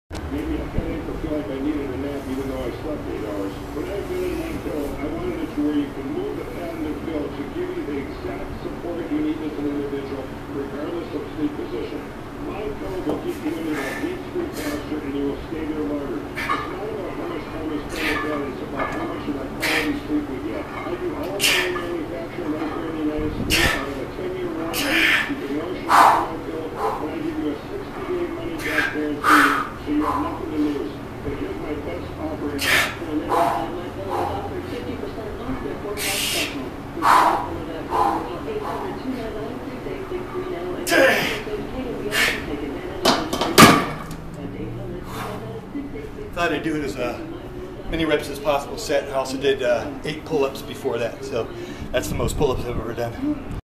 Incline BP 10 X 135 Set 4 of 4
This was a AMRAP set at the end of my Incline BP workout

I also got 8 pullups...that's a big rep PR on those. sorry not vids...